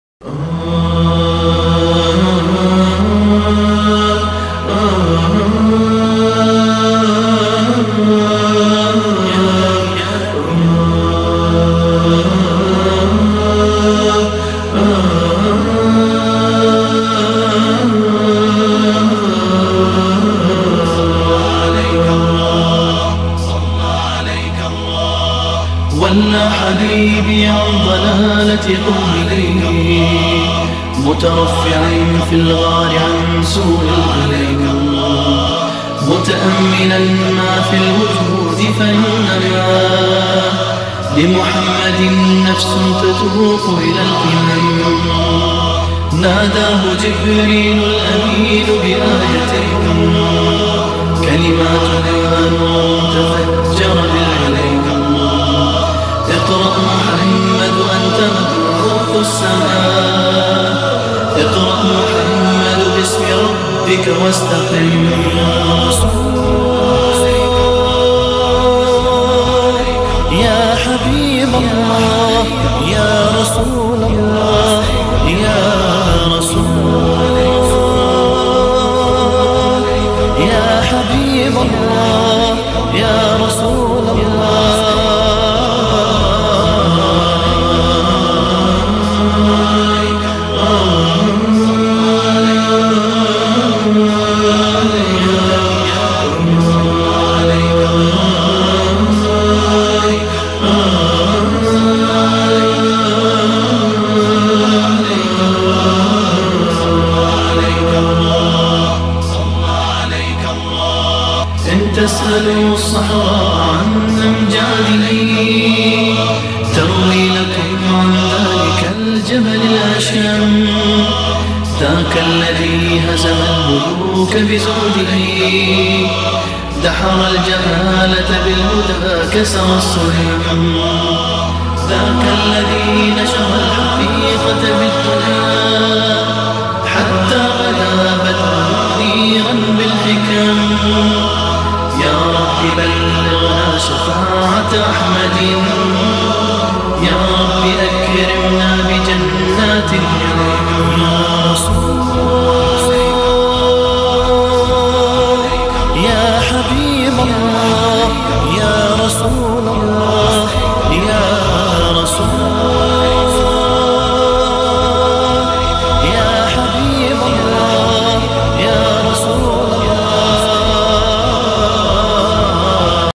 Fetih / Al sancak
Historical Turkish flag with the silhouette of the military army fighting under Constantinople and the figure of Fetih Mehmed, the conqueror.